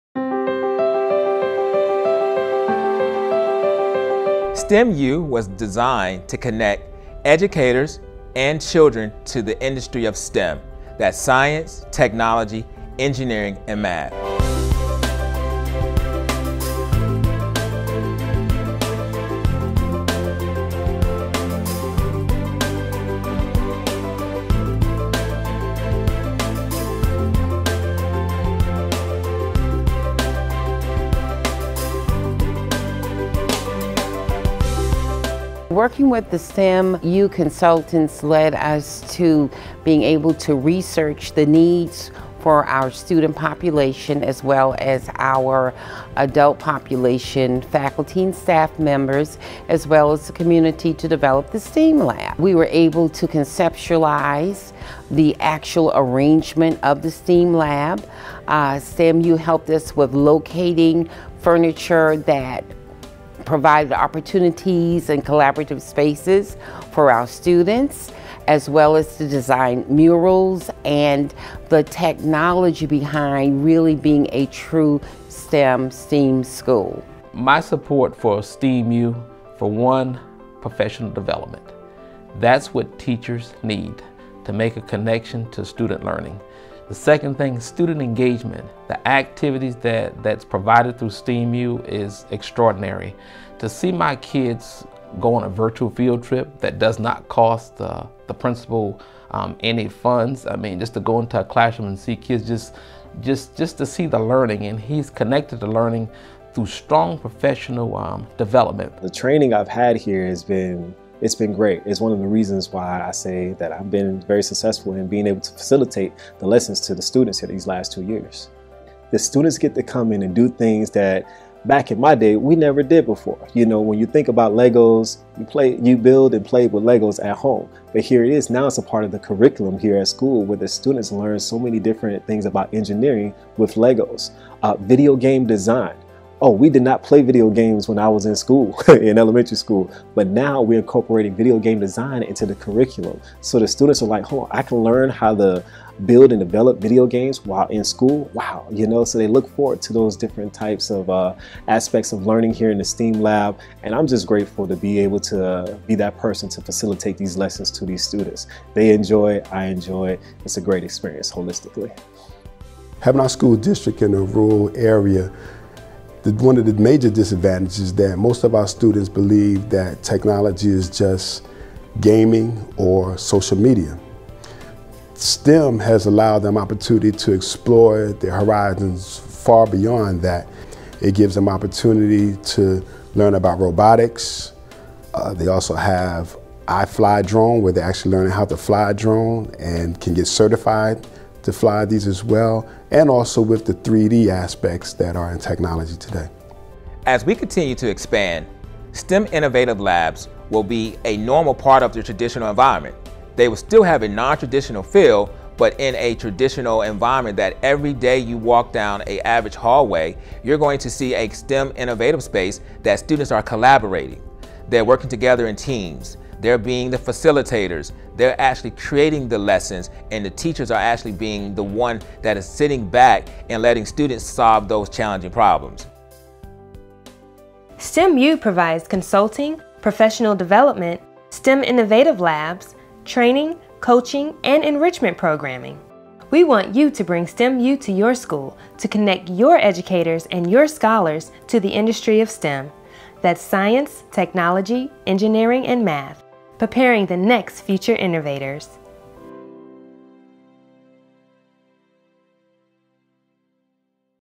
STEM U Promo Video 2019
Grateful for the opportunity to be the Killian Elementary STEAM Lab Teacher, It was pretty cool to be apart of this promo video.